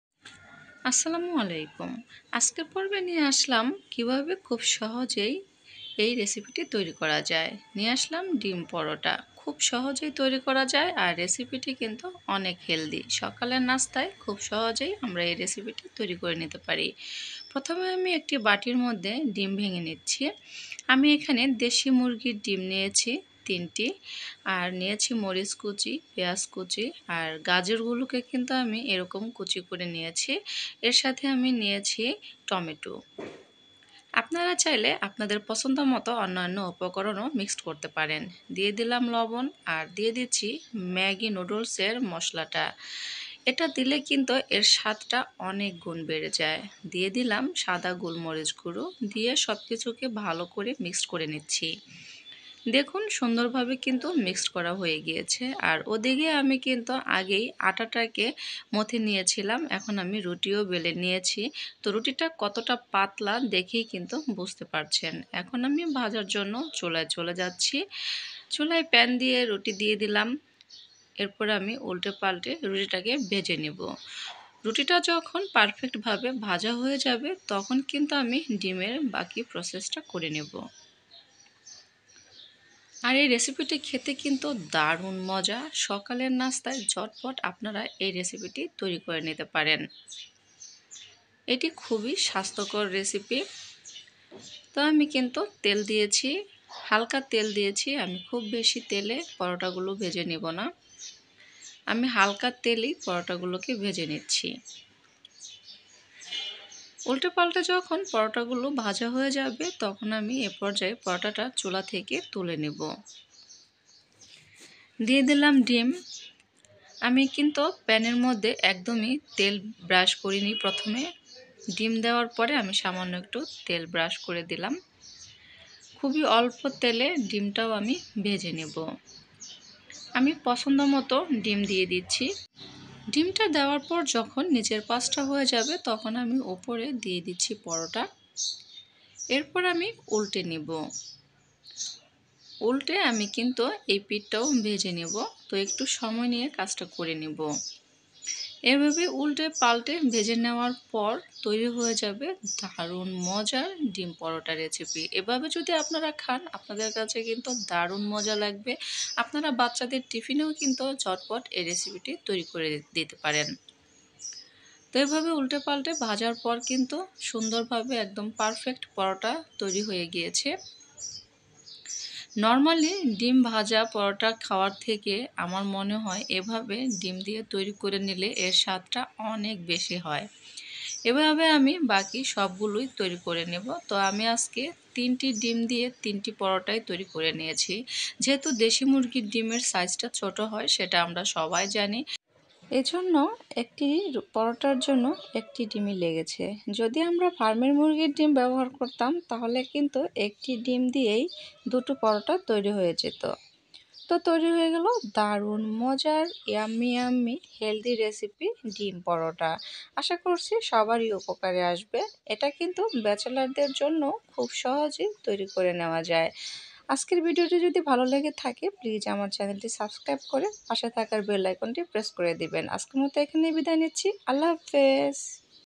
ডিম পরোটা সকাল বিকেলের নাস্তার জন্য সহজ রেসিপি। Easy breakfast recipe! Egg Porota Recipe!Dim Porota.
উপকরণ:
দেশি মুরগির ডিম ৩ টা
টমেটো কুচি
গাজর কুচি
পেয়াজ কুচি
কাচা মরিচ কুচি
লবণ স্বাদমতো
সাদা গোলমরিচগুঁড়া
হলুদ গুড়া
মরিচ গুড়া
ম্যাগি মসলা